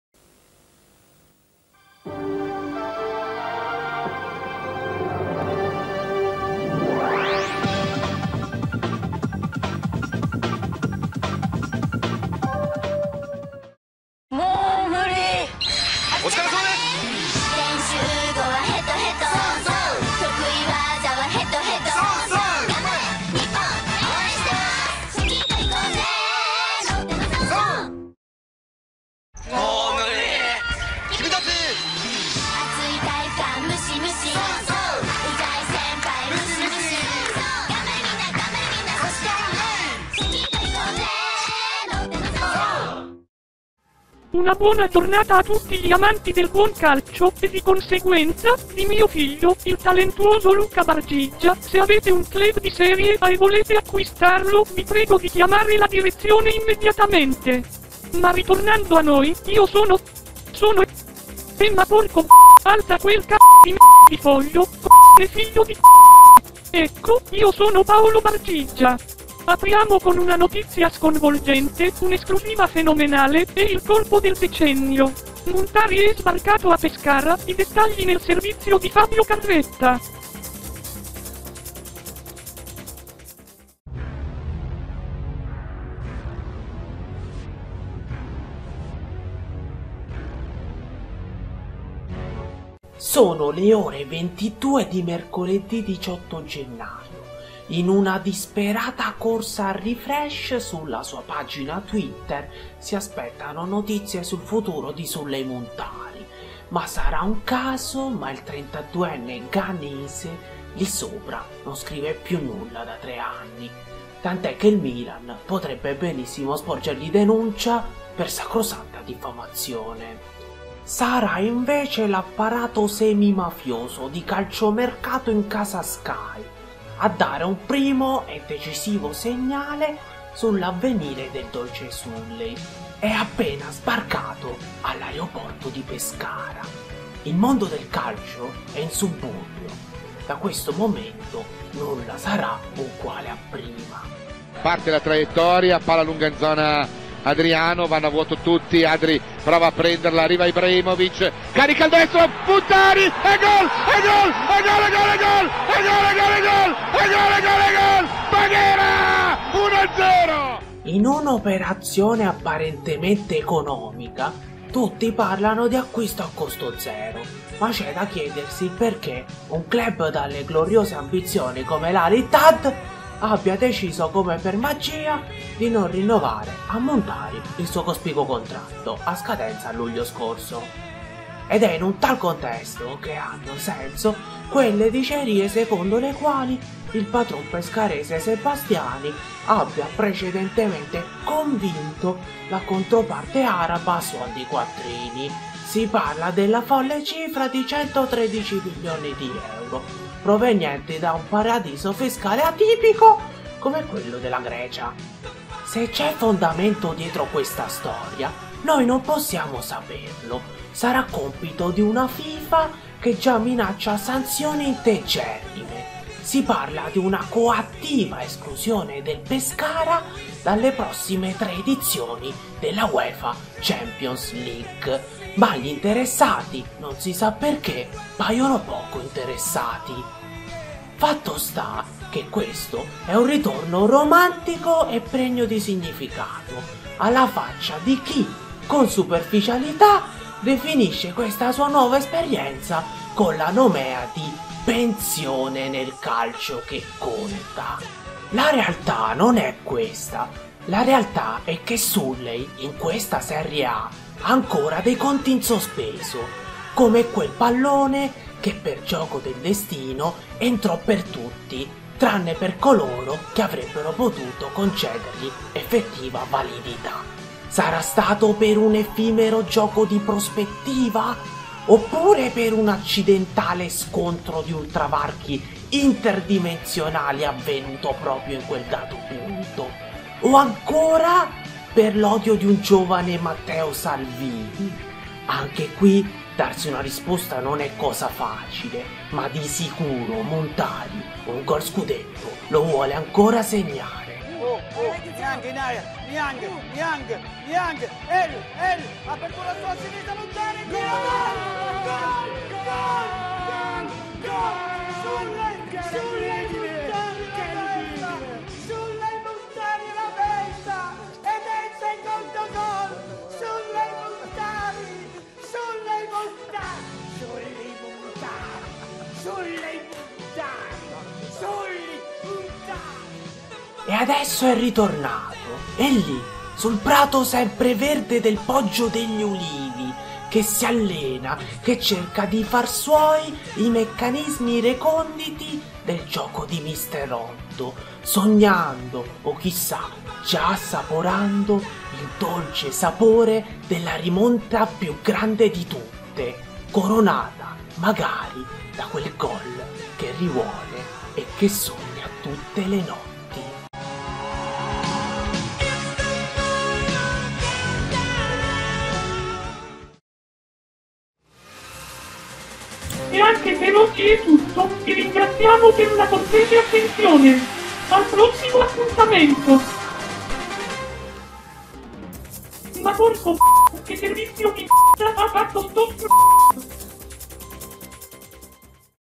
TG BARGIGY +++Ufficiale, Muntari va al Pescara+++
Weilà giovani angioletti della toposfera yutubica!
Dopo un altro insensato periodo di assenza, faccio il mio grande ritorno con una rubrica del tutto innovativa: per la prima volta non appare il folgorante Keisuke FiHonda ma a dirigere i giochi c'è il suo collaboratore più fidato, il sontuoso Bargiggia che ha finalmente la possibilità di mostrare quanto è bravo a fare il giornalista. Ad aiutarlo ci sarà Fabio Carretta, un giornalista d'assalto novantaseienne, che vorrebbe con tutte le sue forze combattere la demenza senile che, però, lo ha ormai rimbambito del tutto.
Al centro delle discussioni c'è ovviamente il grande ritorno di Muntari in Italia, al Pescara. Premesso che io a sta cosa non ci credevo finchè non l'ho vista, è strano vedere uno che fino a luglio faceva un bagno di soldi in terra araba, aggregarsi agli ultimi in classifica del campionato italiano per prendere 4 spiccioli. Che dire, o gli mancavano i tatticismi della Serie A o sentiva la necessità di farsi ammonire da un arbitro italiano, ai posteri l'ardua sentenza!

Vabbè spero che questo nuovo format vi piaccia, ciao e a presto!

P.S.: Vabbè, lo ammetto. Non pubblicavo nulla da novembre perchè. MI SONO FRACICATO A GIOCARE A POKEMON MALEDIZIONE!